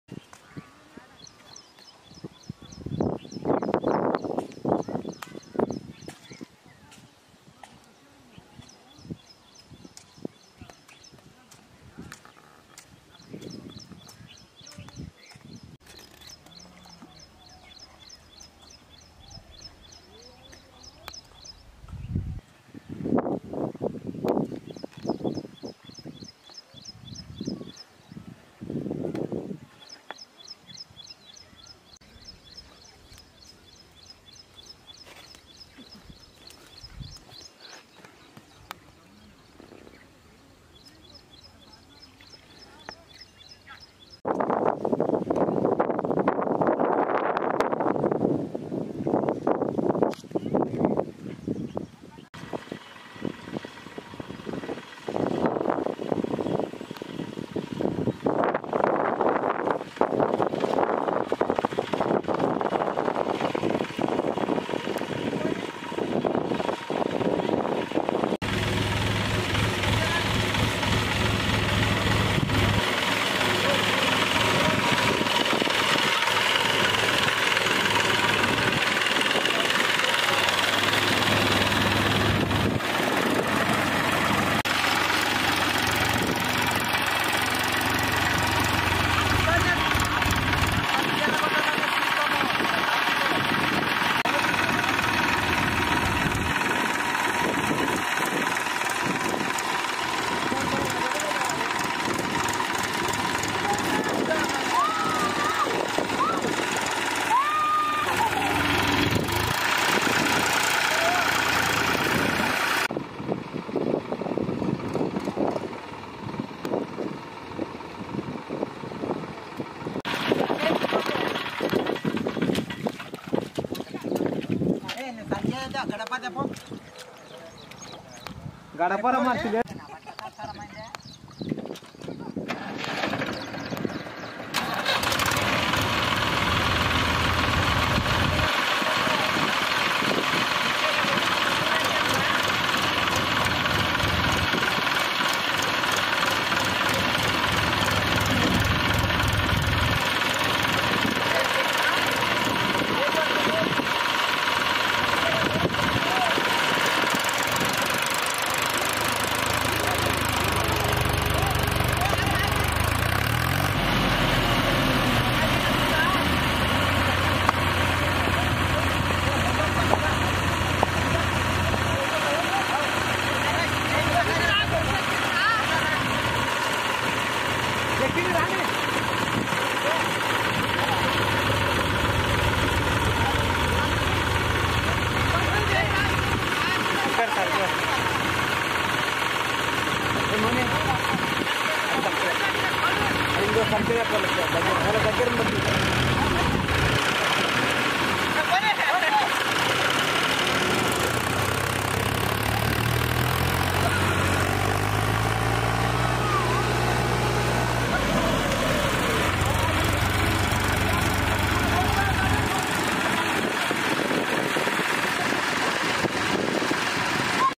Mahindra tractor stuck in mud Rescued by Massey Ferguson tractor |tractor videos
Mahindra tractor stuck in mud